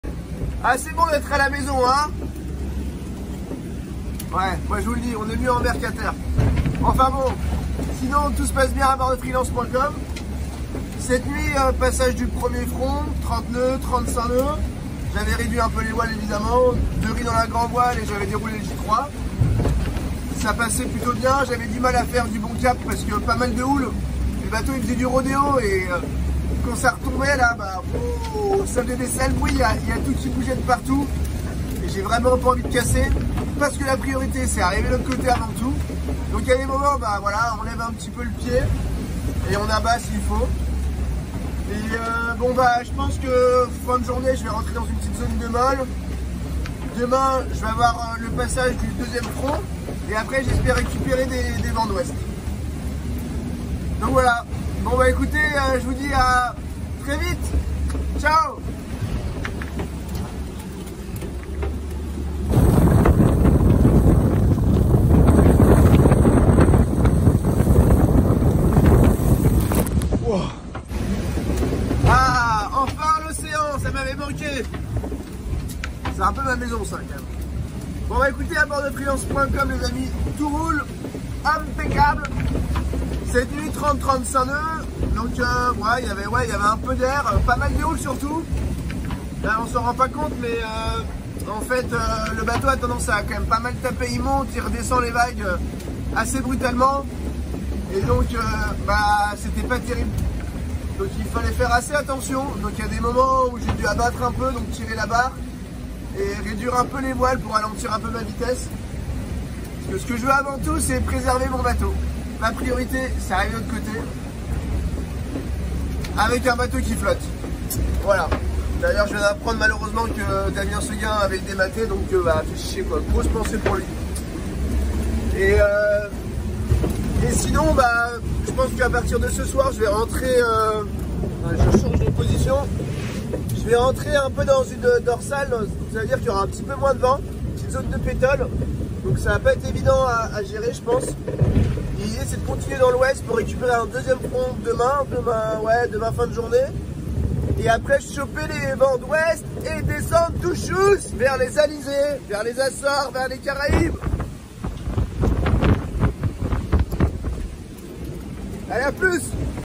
Day 3 onboard Freelance.com (Guirec Soudée) | Route du Rhum
🔴 Vendée Globe 2020
🔜 The Ocean Race 2022